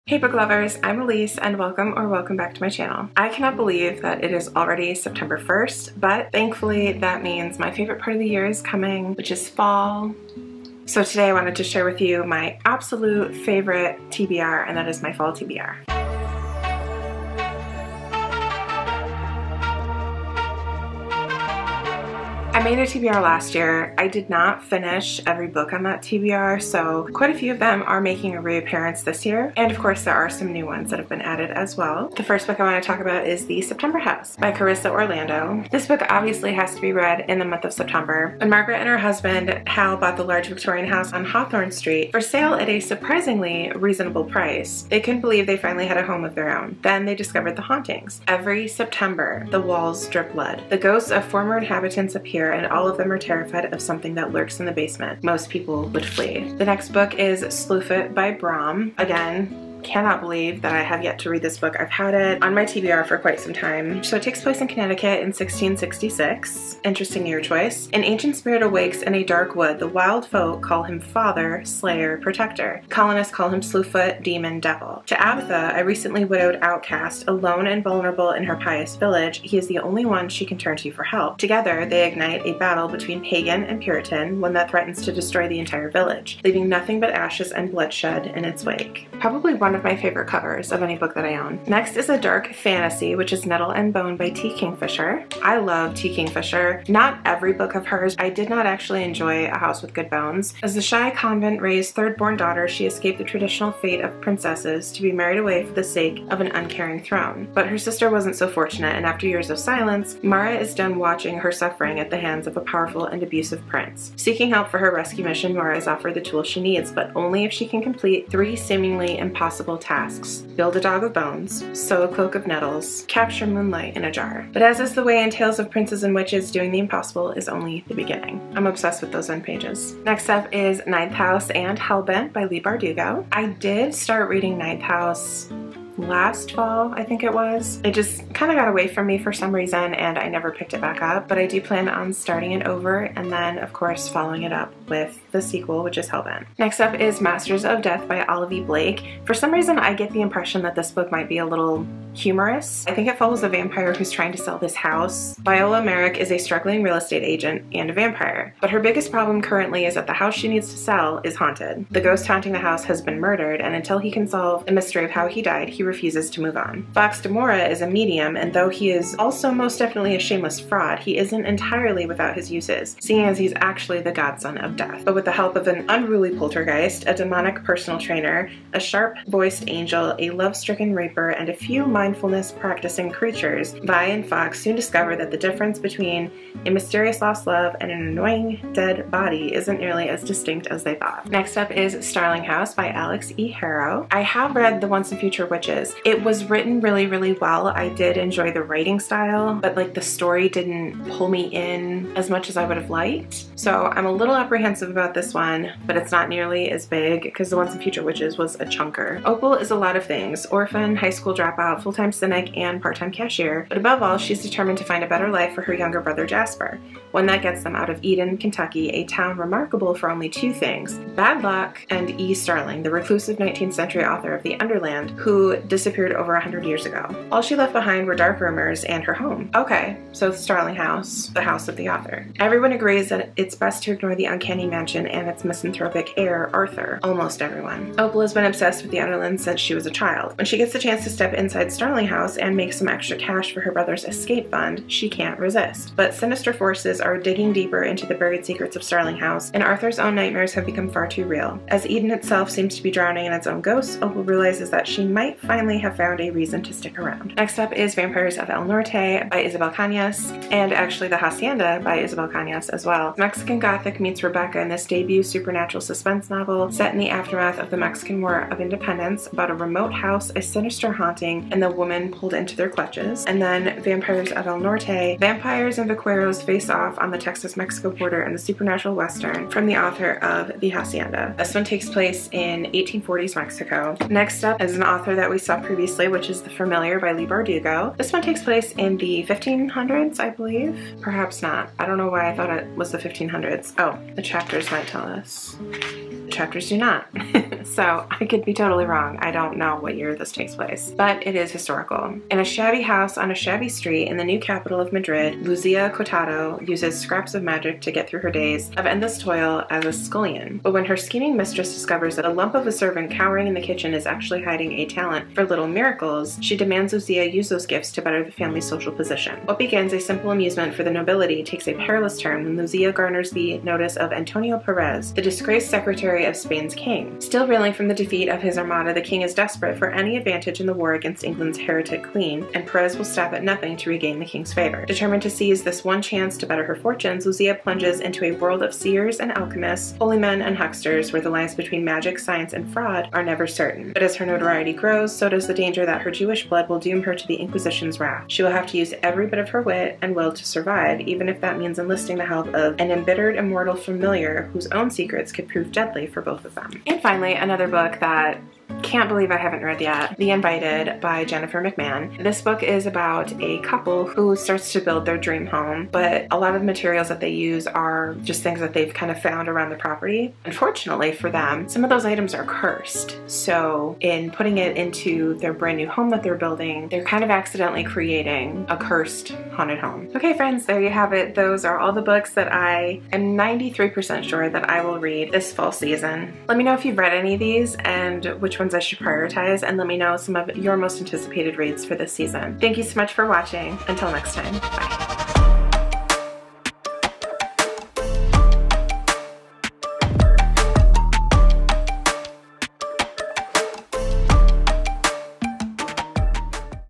Fall TBR 🍁📖 horror & dark fantasy
All the spooky reads I WILL get to this fall!

Like SciFi? Join my science fiction book club! ✨

Amazon Wishlist 💖